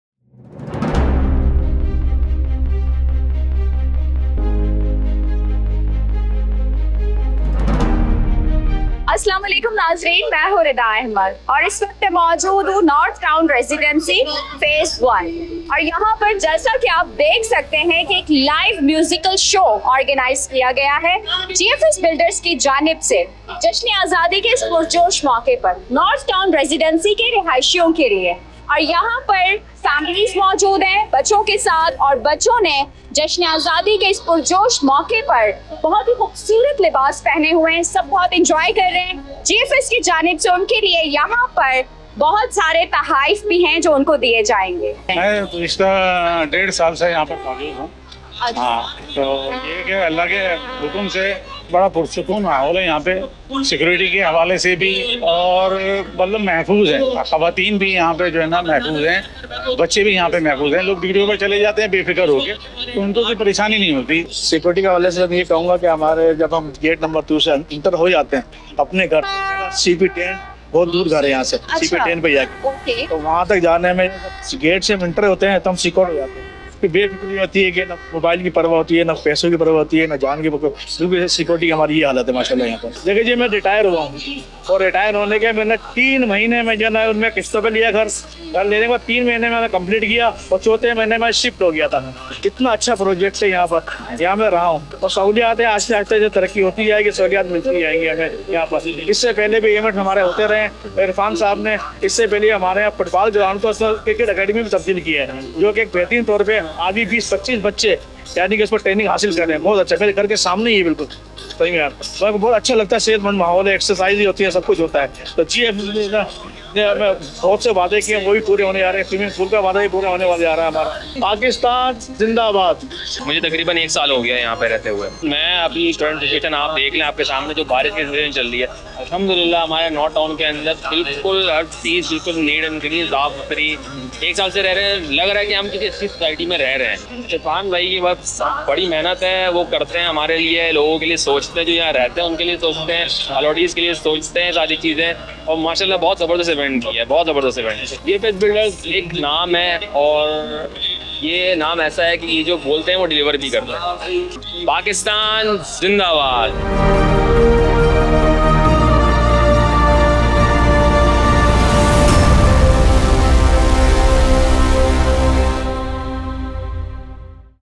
GFS Independence Day Gala & Live Music at North Town Residency Phase 1
GFS Builders & Developers celebrated Pakistan’s Independence Day with an extraordinary gala, showcasing a live musical performance specially crafted for the North Town Residency community. 🇵🇰🎉

The event dazzled with its unique flair, as NTR Phase 1 residents and their families enjoyed a night filled with joy and engaging activities. Attendees were thrilled by the festivities and praised GFS for its commitment to delivering premier features and amenities while ensuring top-notch security within the North Town Residency project.